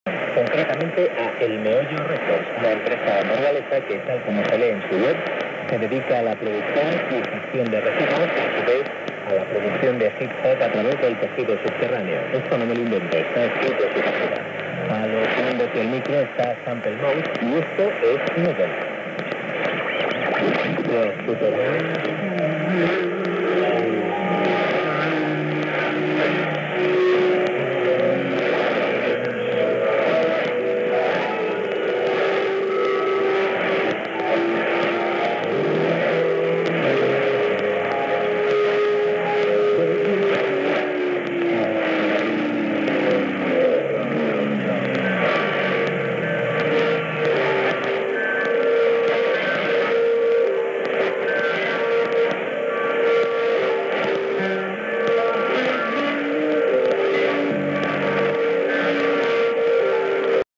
ree [200kw, noblejas] — 17855 khz — [26 mar. 2017 21.24 utc]
Local Time: UTC +13H
Frequency: 17855 kHz
QTH: New Zealand, Hira
Ant.: LW more or less 80 metres
RX: SONY ICF SW-77